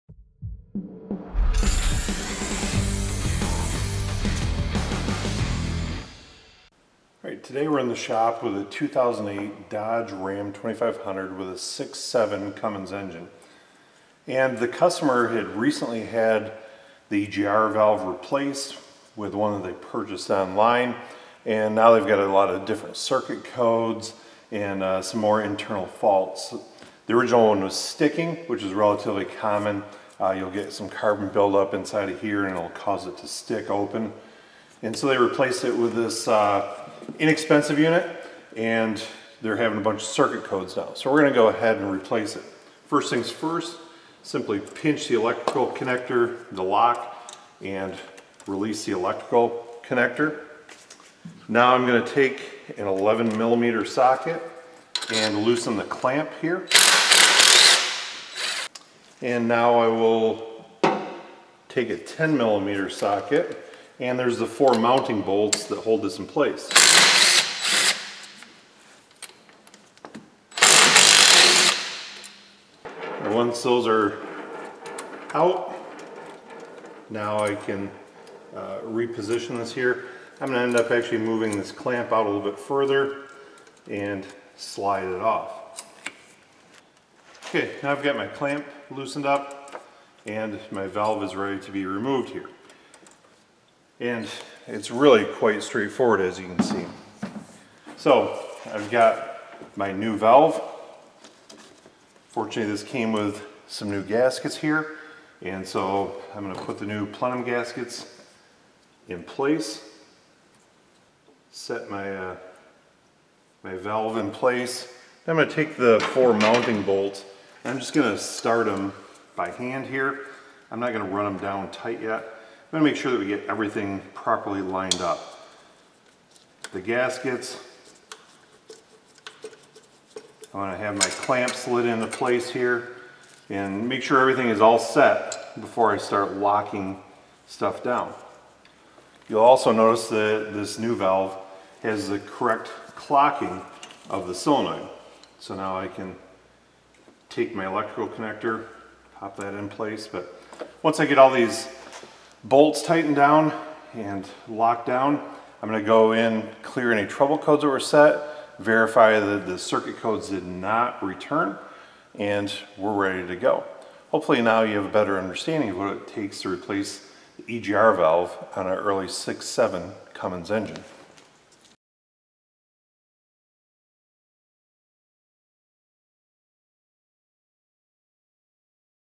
Cummins 6.7 EGR Valve Replacement | Installation Spotlight
Today we're in the shop with a 2008 Dodge RAM 2500 with a 6.7 liter Cummins engine. The customer recently replaced the EGR valve with one they purchased online and now they've got a lot of different circuit codes and some more internal faults. We're going to replace it.